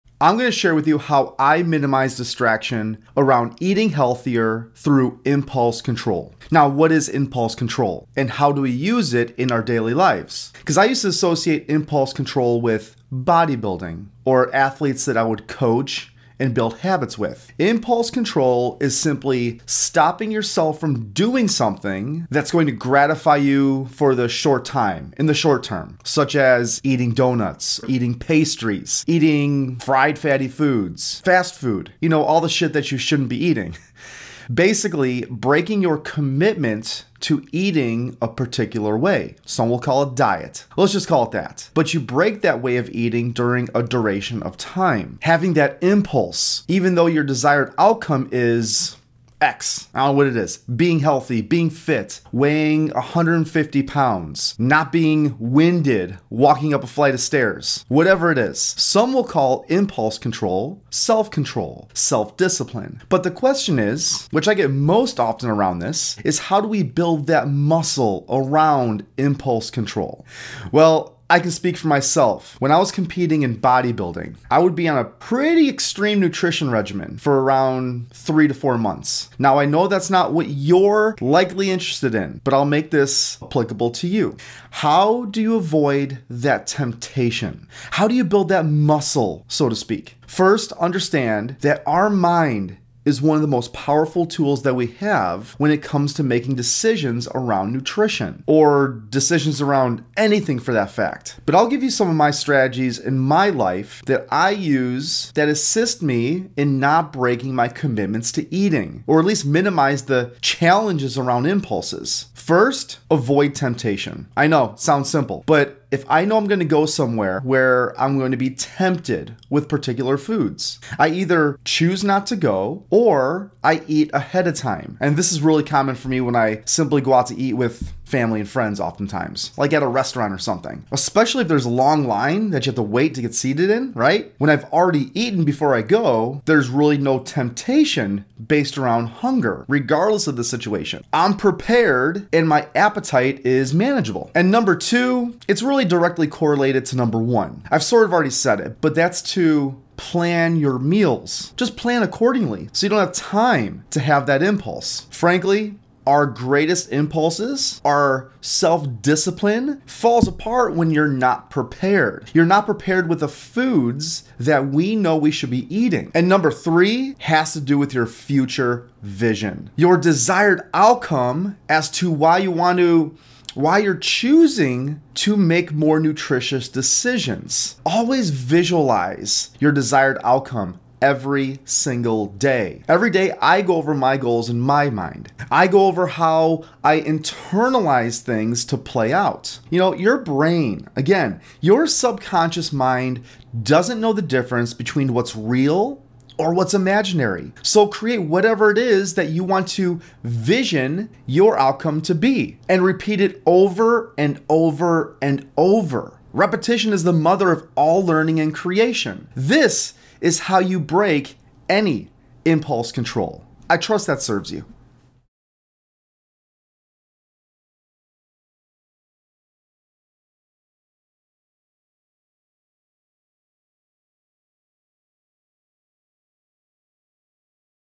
How to manage your Impulse Control while dieting
I'm grateful to share my experiences in how I manage Impulse Control around eating so that YOU stay on track with your desired outcomes too.
If you fail to plan, then plan to fail.
When your vision is clear, so are your life choices.